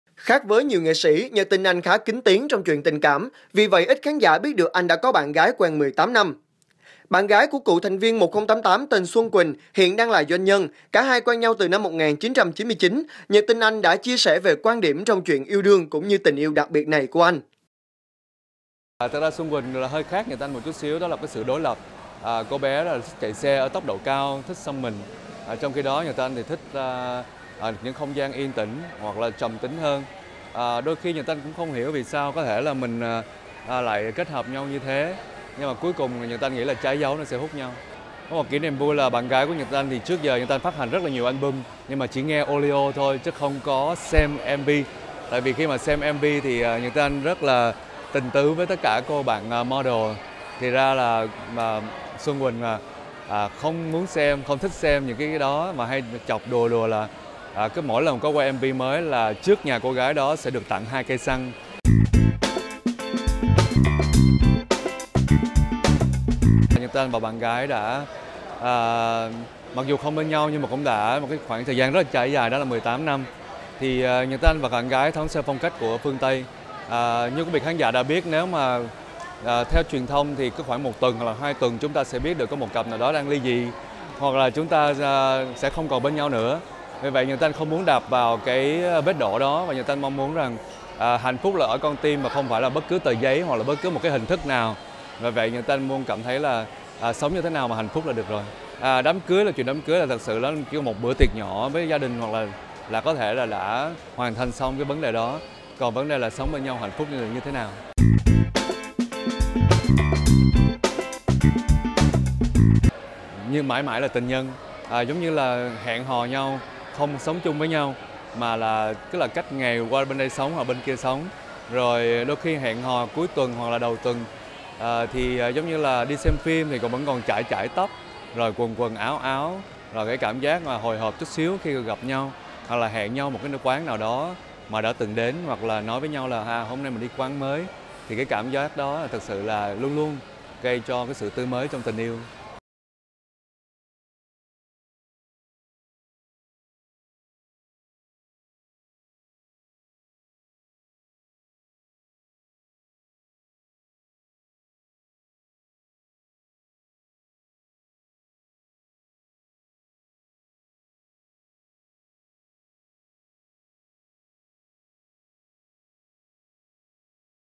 Người yêu Nhật Tinh Anh đòi đốt nhà người mẫu diễn chung với bạn trai
Khác với nhiều nghệ sĩ, Nhật Tinh Anh khá kín tiếng trong chuyện tình cảm. Vì vậy ít khán giả biết được anh đã có bạn gái quen 18 năm.